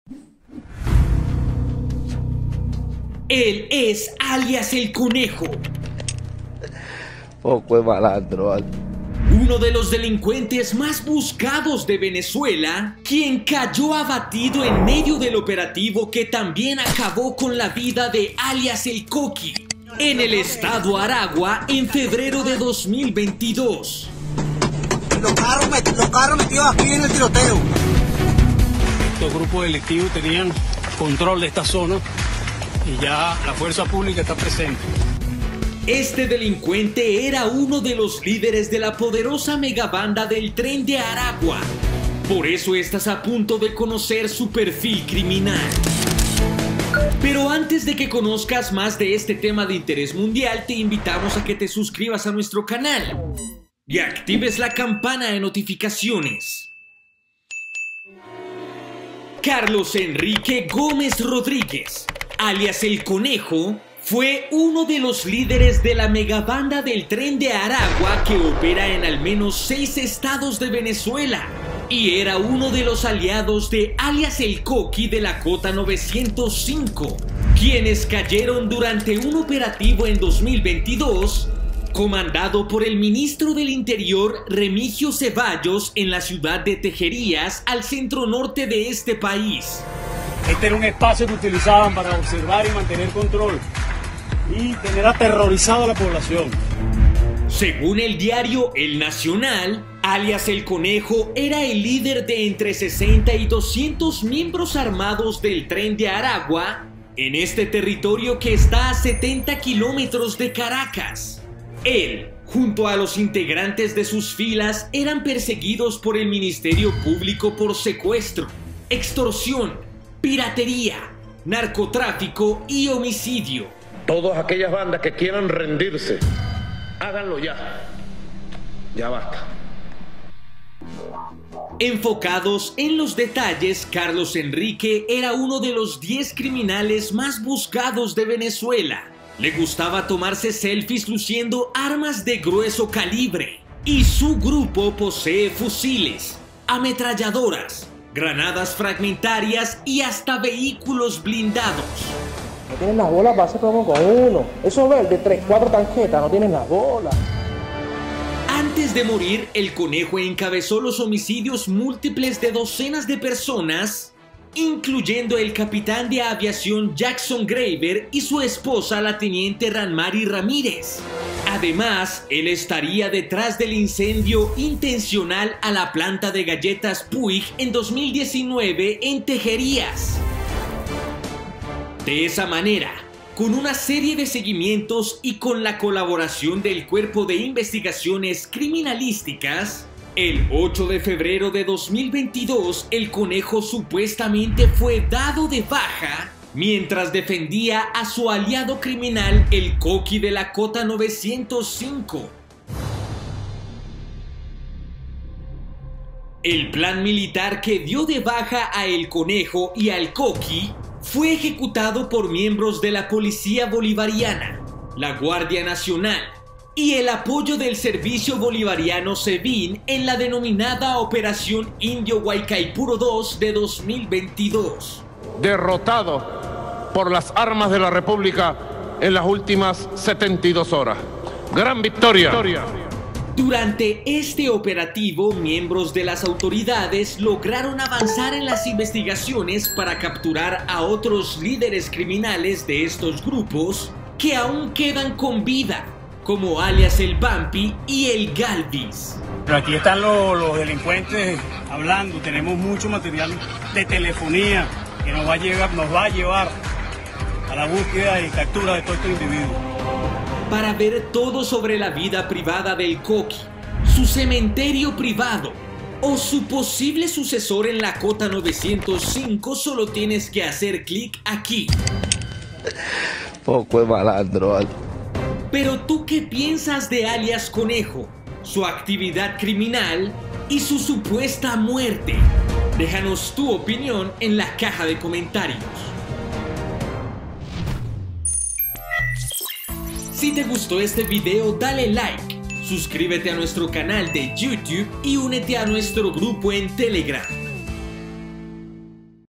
ALIAS EL CONEJO ¡aliado del KOKI! PERFIL CRIMINAL I Impacto Mundo
Un criminal de una gran reputación ilegal y líder de la poderosa megabanda del Tren de Aragua, fue alias El Conejo. Este personaje cometió una gran cantidad de crímenes y se alió con el también abatido delincuente alias El Koki de la Cota 905. En Impacto Mundo investigamos su vida, para traerte su ¡Perfil Criminal! Periodista: Jorge Barrera

🎥 Nota recomendada: ¡MUERTO! Asesinaron a EL KOKI

🎥 Nota recomendada: Perfil Criminal - Tren de Aragua